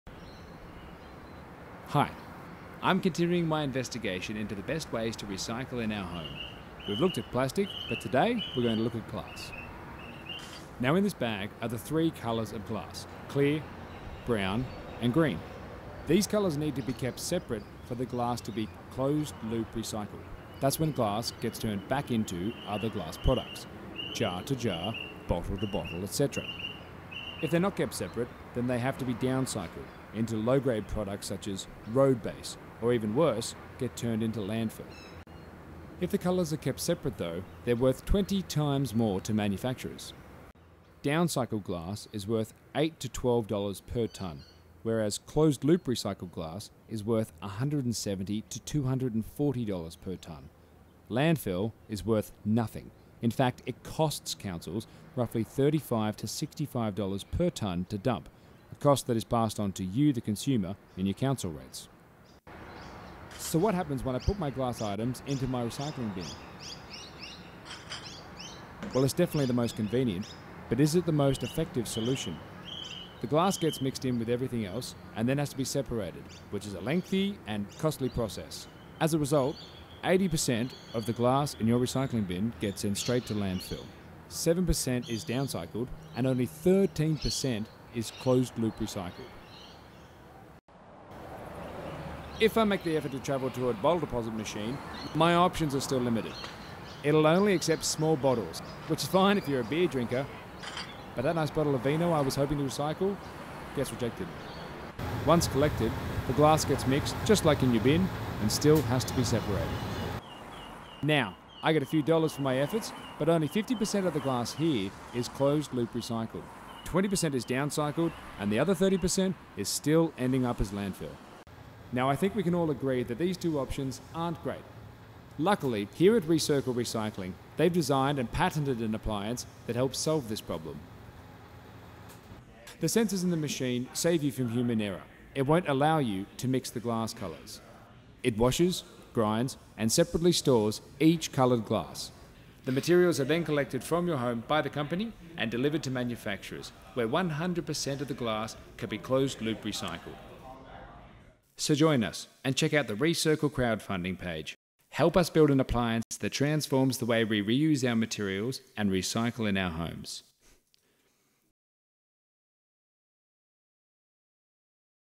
Glass: How much is your recycling worth?
Council pick-up? Or bottle deposit machine? ReCircle’s Daniel Milne looks at the best way to recycle glass.